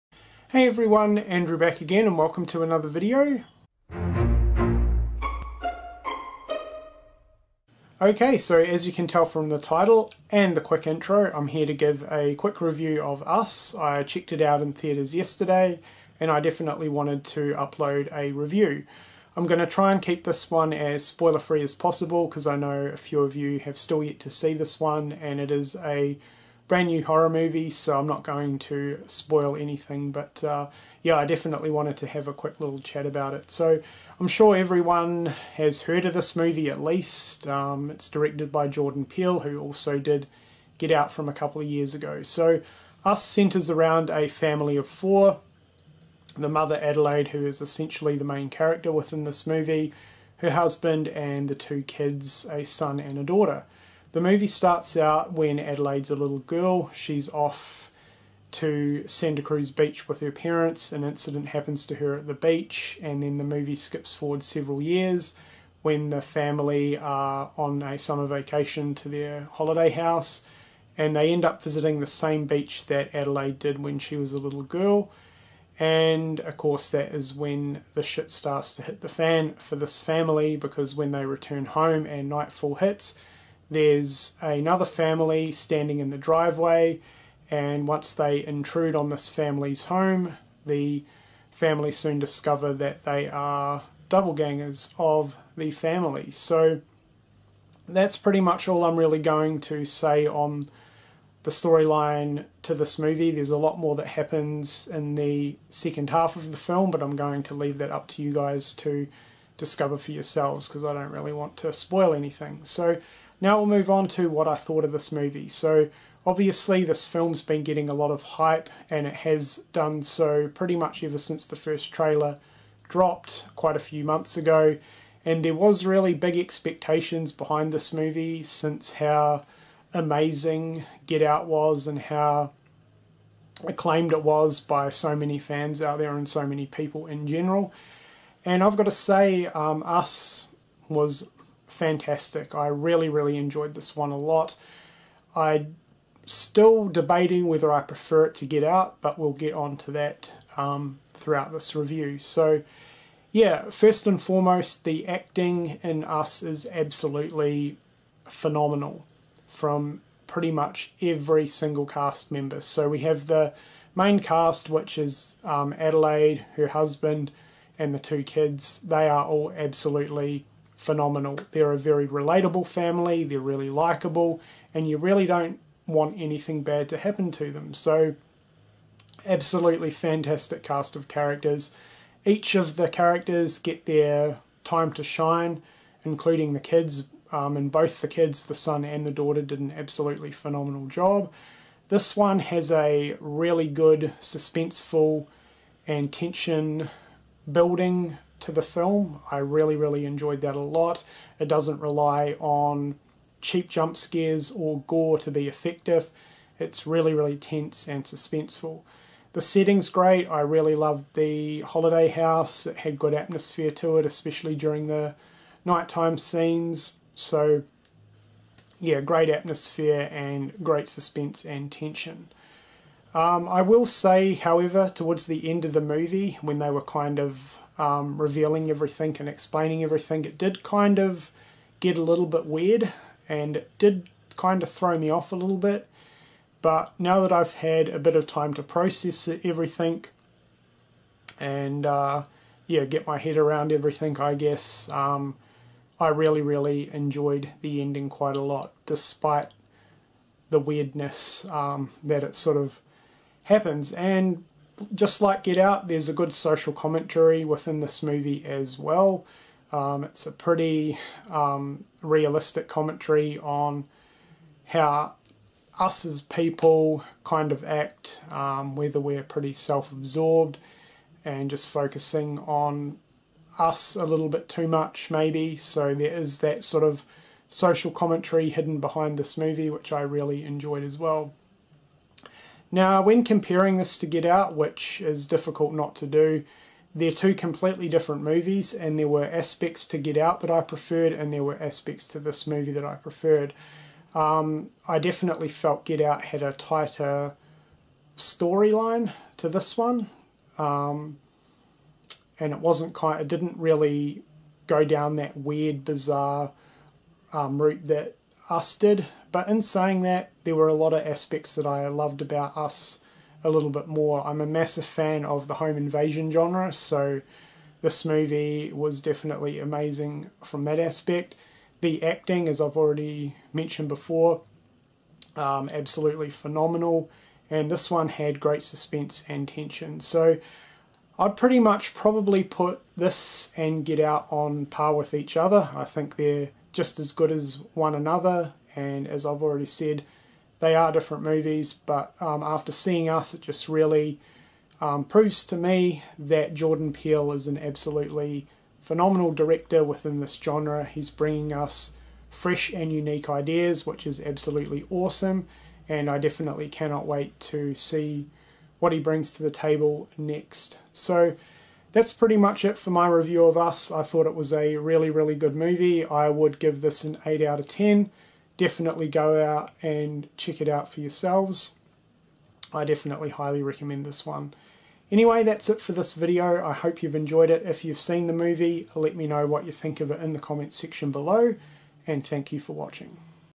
Us (2019) Movie Review
I'm here with a review of Jordan Peele's, Us.